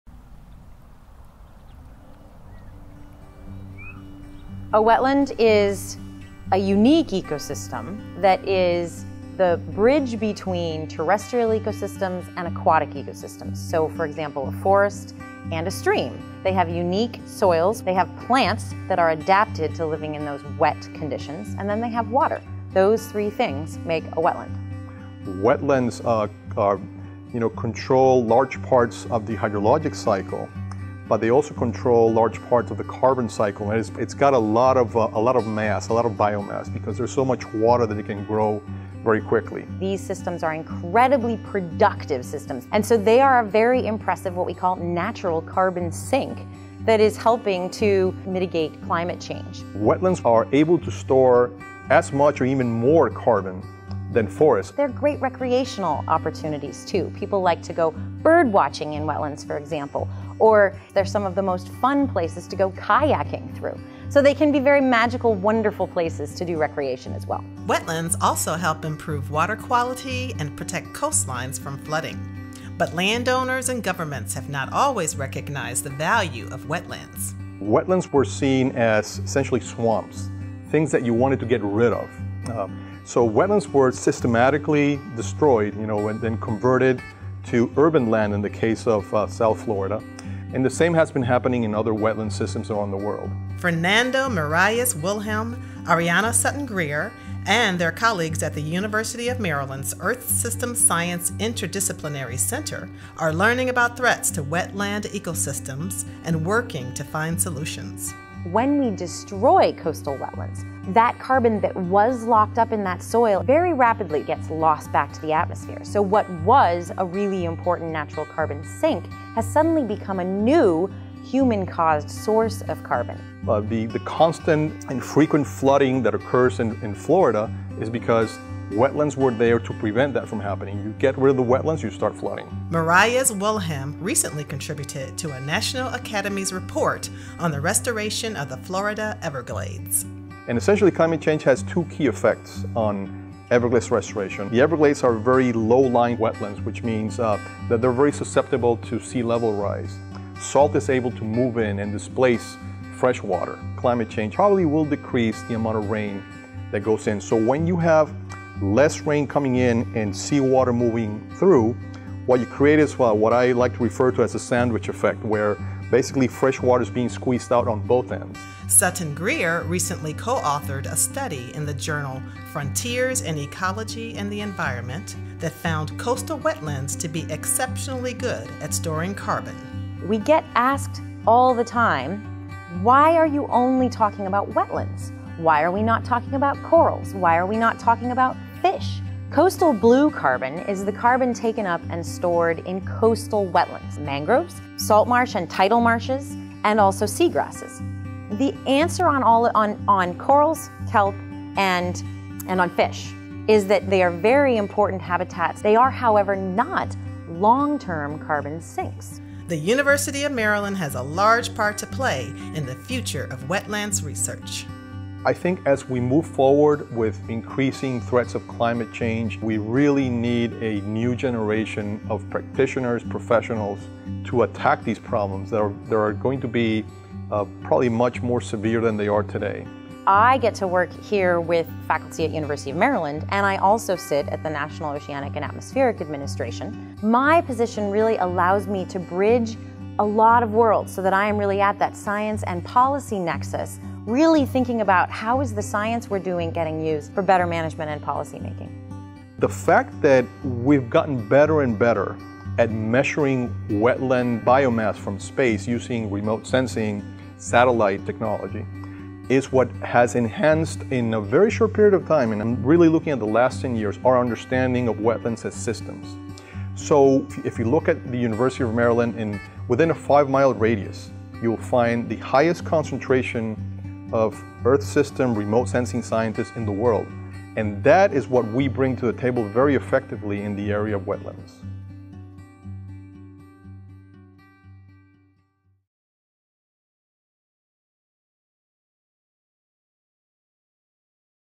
The Secret Life of Wetlands: Finding Solutions to Environmental Threats at UMD
Wherever land and water comes together, you are likely to find a wetland. These complex ecosystems provide habitat for wildlife, protect our coastlines from flooding, store massive amounts of carbon in their soils, and offer great recreational opportunities. Fernando Miralles-Wilhelm, Ariana Sutton-Grier, and their colleagues at the University of Maryland's Earth System Science Interdisciplinary Center (ESSIC) are learning about threats to wetland ecosystems and working to find solutions.